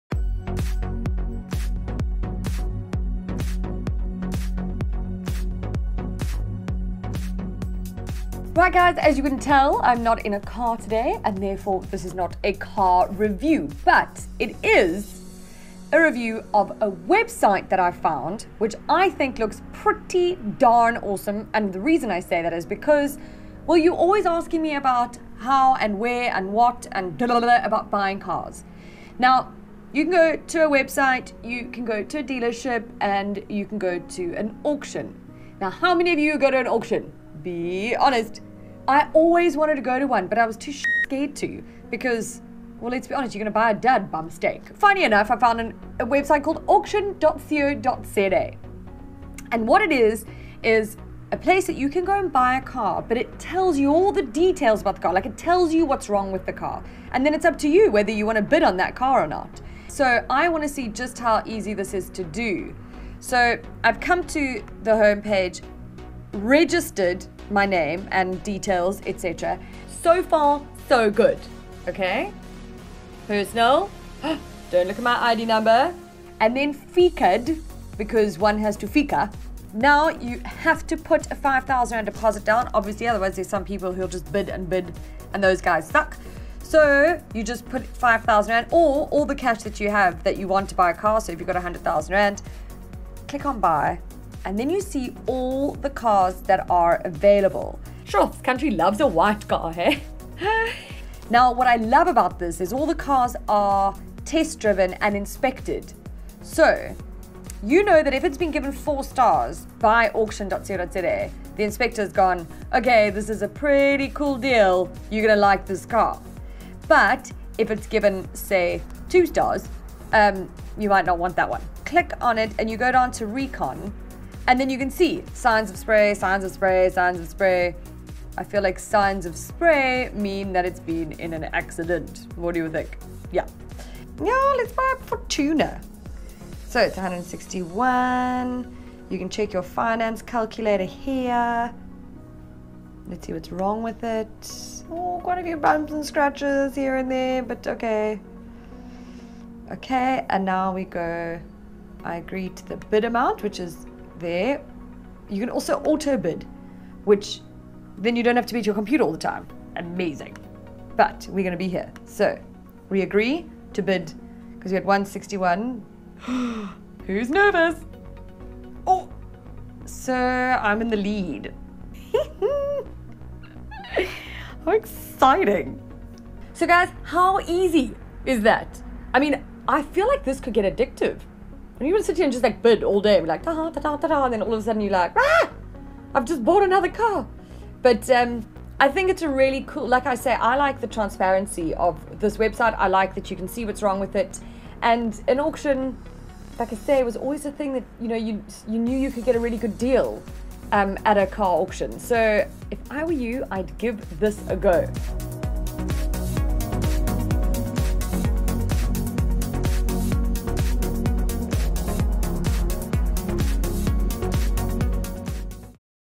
Would you buy a car through an auction?
I came across this website called Auction.co.za and having always been intrigued by car auctions, I decided to give it a go. On camera! Just for YOU!